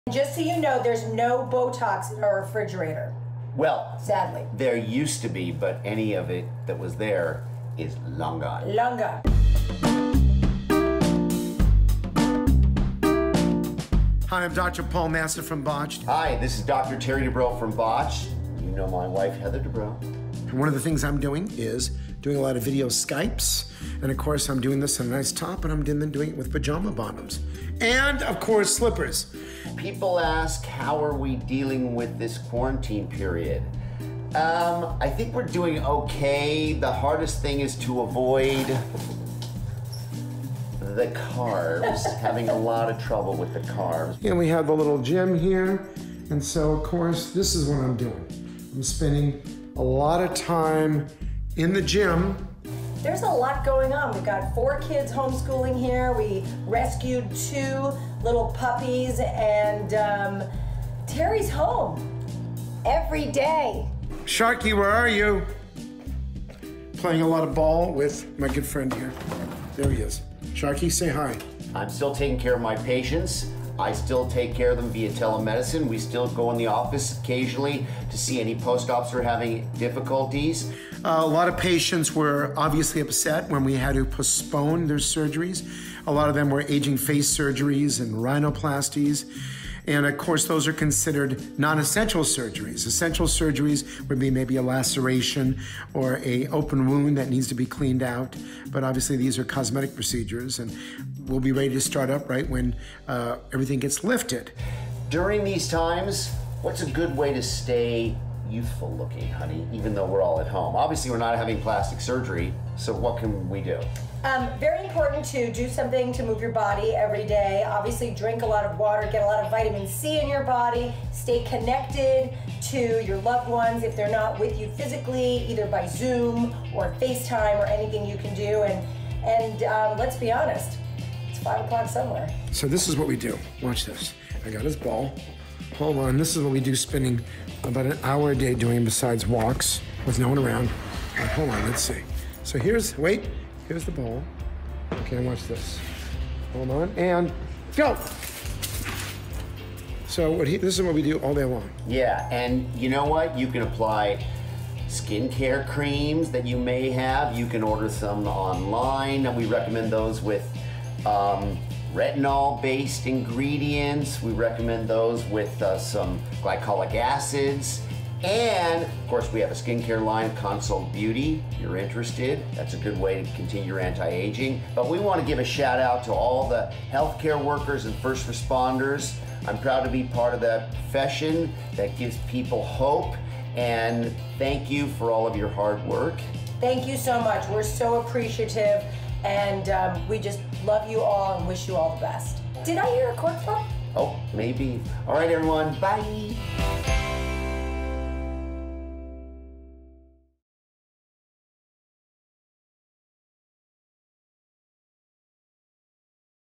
How the 'Botched' Doctors handle plastic surgery patients from home | Page Six Celebrity News
The “Botched” doctors may not be able to perform any surgeries, but they’re staying busy while quarantined. Dr. Paul Nassif and Dr. Terry Dubrow — alongside his wife, former “Real Housewives of Orange County” star Heather Dubrow — shared exclusive home videos with Page Six. Nassif revealed that he’s been doing plenty of Skype consultations while in his pajama pants. Dubrow is busy avoiding carbs and Heather revealed that all the botox from the fridge is gone now. The couple rescued two puppies and they’re homeschooling their four children as well. Both men will most likely be watching “Botched” when it returns to E! Mondays at 9 p.m. ET.